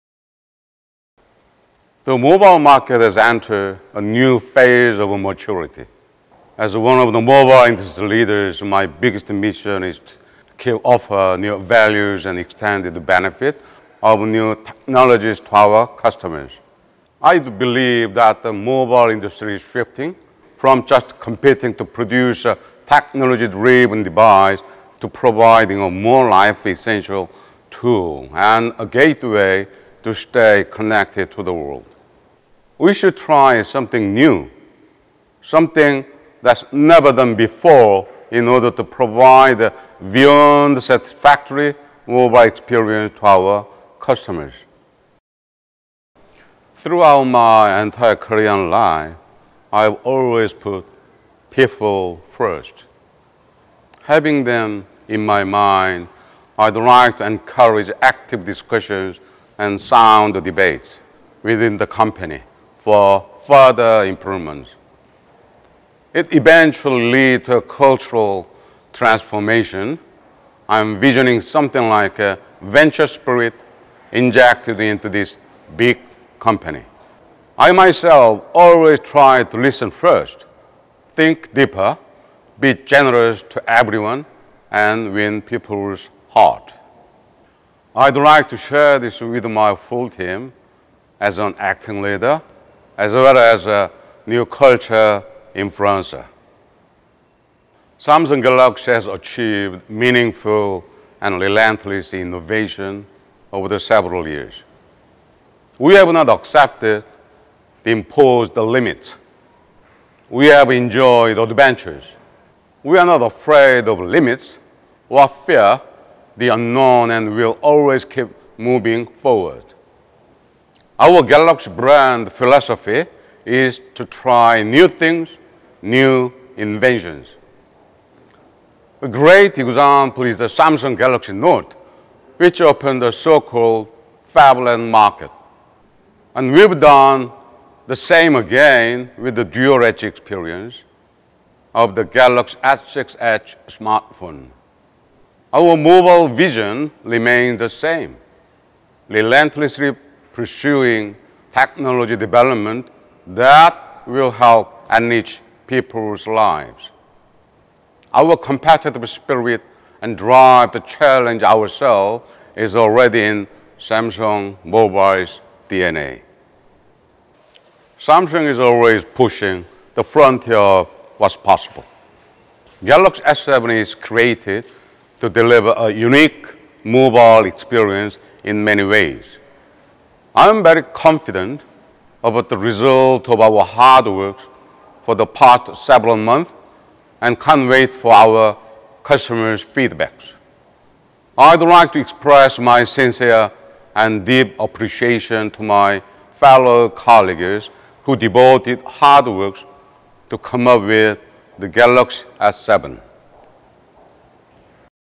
الرئيس Dongjin koh يتكلّم حول رؤية قسم الهواتف المحمولة من سامسونج قبل حدث UNPACKED 2016
الرئيس Dongjin koh يناقش رؤية الشركة والمنتجات الجديدة والأهداف الشخصية قبل حدث سامسونج Unpacked 2016.